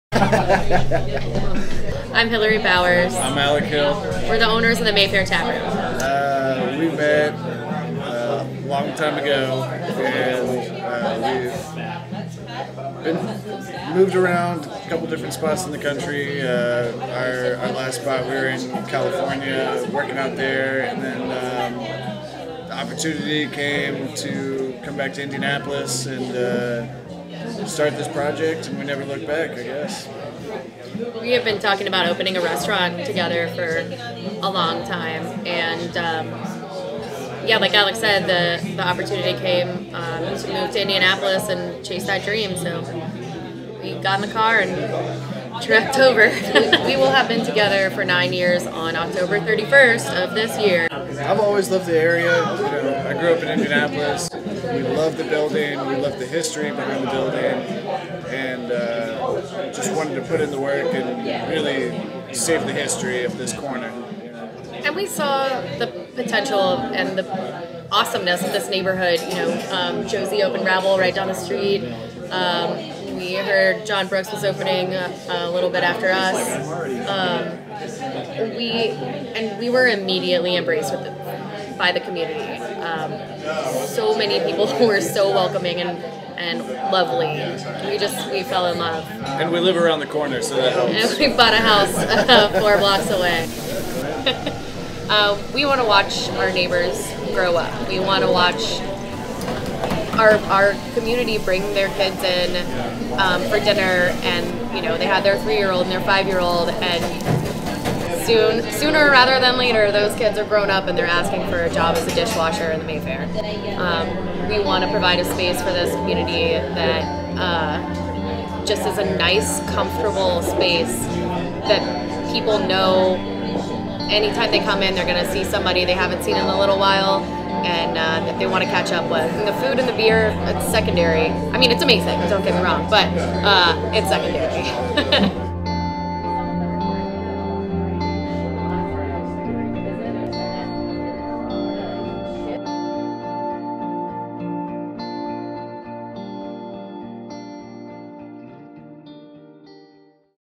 Mayfair Taproom
Recently opened Mayfair Taproom brings history to the forefront with its English & American fare in a remodeled, cozy space that dates back over 120 years